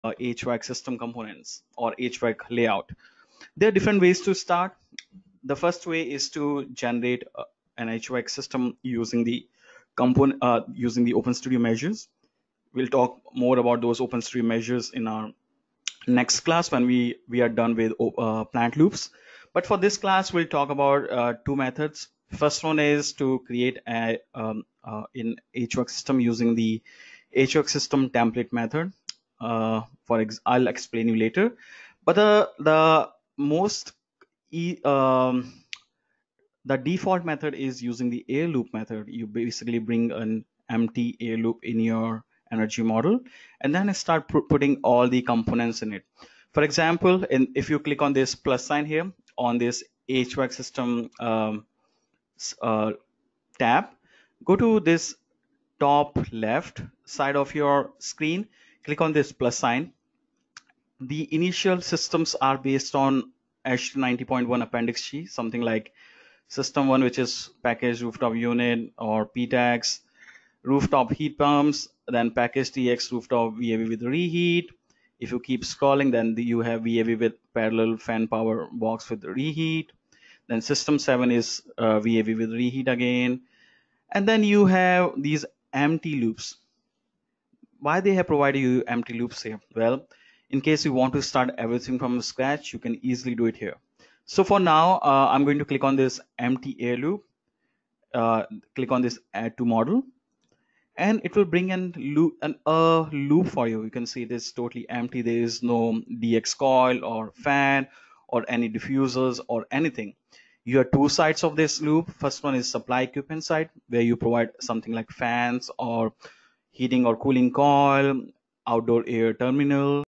How to Model Air Loops in OpenStudio
Thanks for viewing this preview video from Energy-models.com. We hope it is helpful! To access the full video please sign up for the course

Hi everyone. This is Harshul Singhal, again. For our last class we talked about the zone equipment. We talked about different kinds of systems, like package terminal heat pumps,PTACS, the zone part of fan coil unit systems, and a lot of other information, okay. For this class, for today,we'll talk about air loops. There's a lot of information on HVAC that we need to provide and that's one of the reasons why we decided to segregate this HVAC component into multiple classes. We don't want to give you more information on plant loops today, because, again, you will get confused. And, that's why we wanted to, we wanted you to understand the difference between air loops and plant loops, okay. Let's move forward. I'm going to create some air loops today, they're different ways to do that.